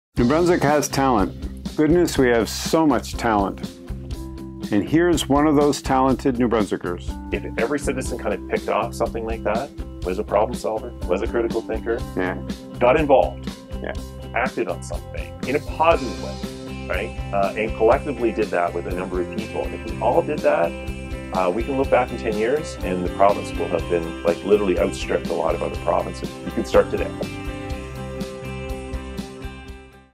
Upcoming Episode: David Alston - Innovation, Drive and Heart (Business)
David Alston's path to this moment in time is exceptional. Mainstream media has told you about the results - the success of Radian6. But they report only what happened ... not how it happened.

This wide-ranging, deep, fascinating conversation is an example of how the entrepreneurial soul moves through a day or addresses a challenge. It is also an explanation as to how to make things happen ... how to initiate change.

The conversation is also a lot of fun.